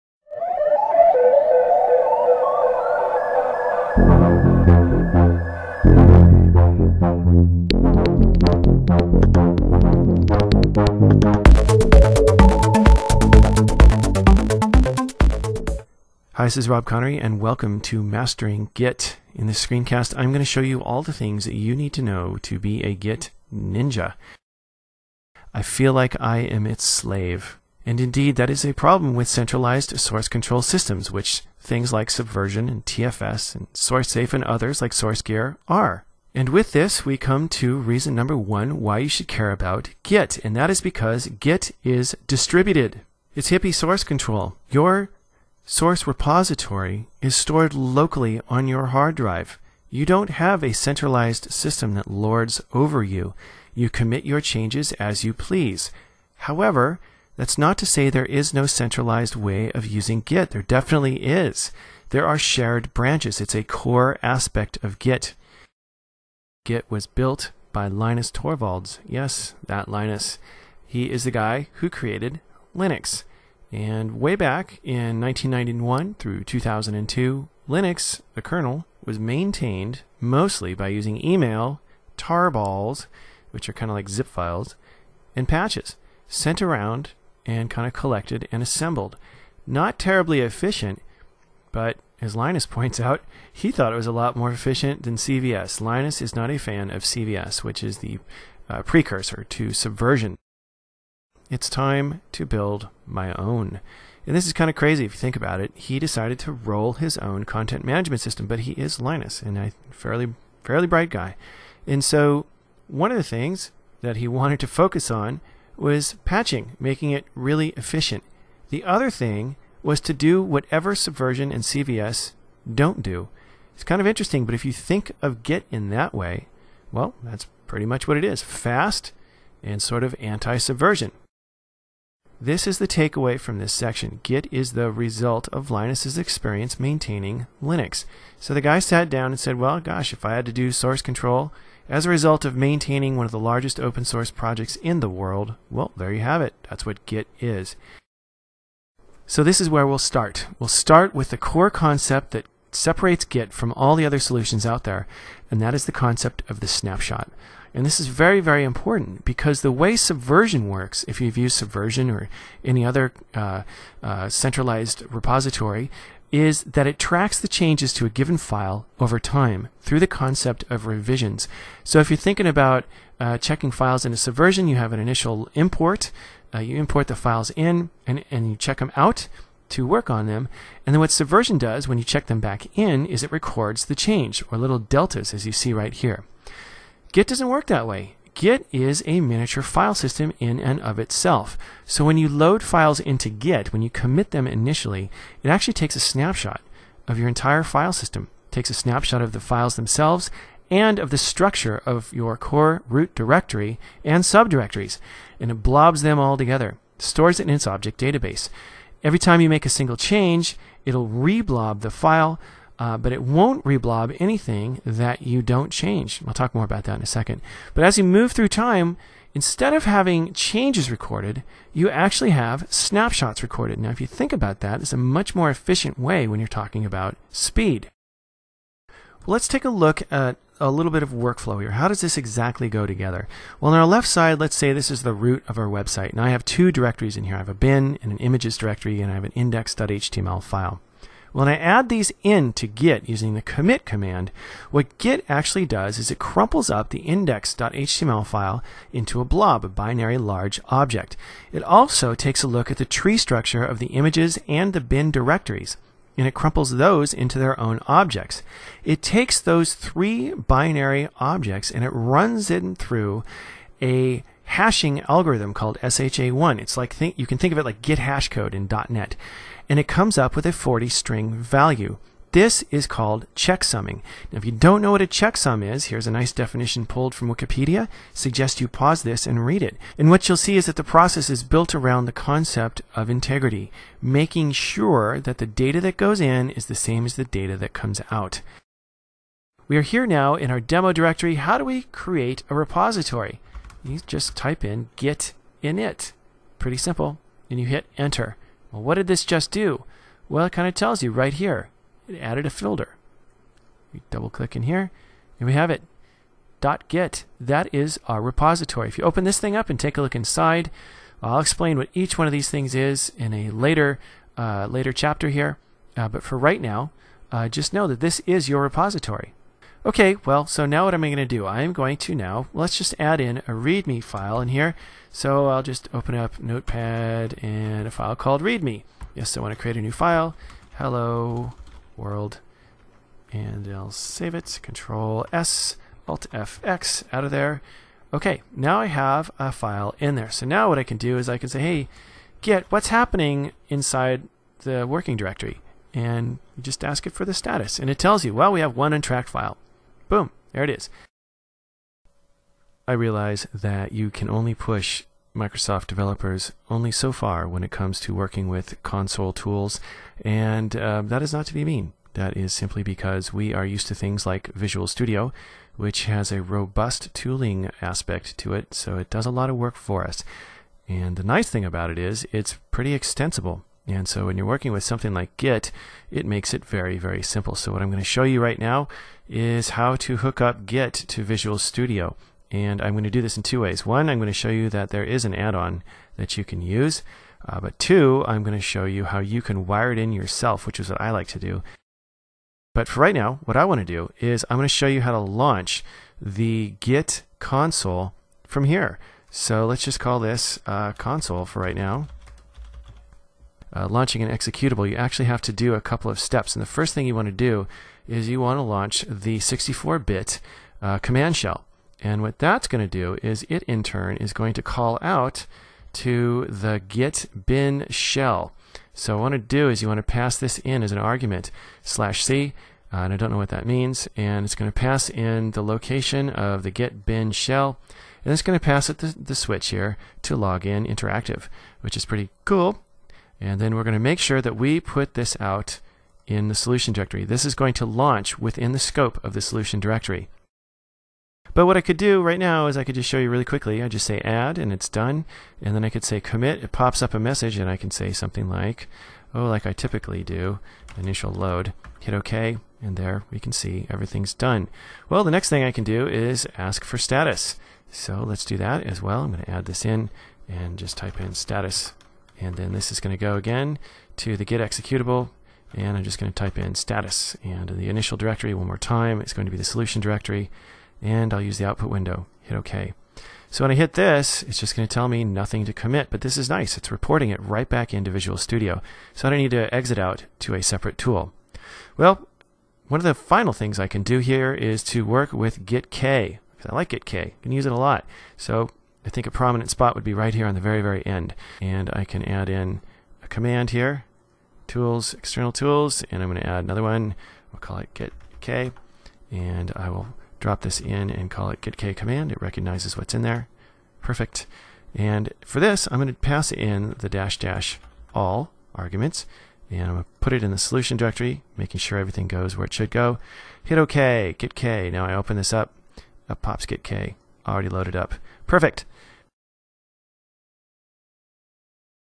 Tekpub: Mastering Git
Mastering Git takes you deep into Git to give you the working knowledge you need to confidently use this system. Git is an amazing source control system and will change the way you work - we've devoted over 3 hours and hopefully you'll agree that Git's rockin.